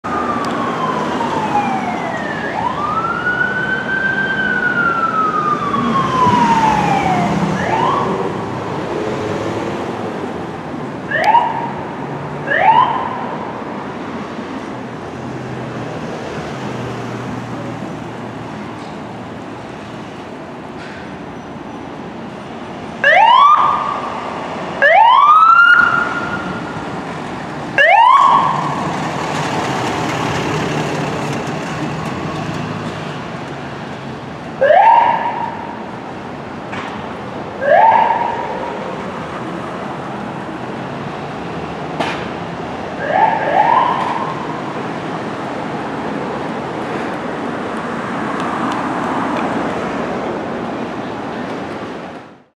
LAFD Rescue 9 Responding 🤙
Los Angeles Fire Department Rescue 9 responding and arriving on scene of a medical call in their first in. Thanks to the paramedic for the double hang loose!

Downtown, Los Angeles, CA - 2/9/21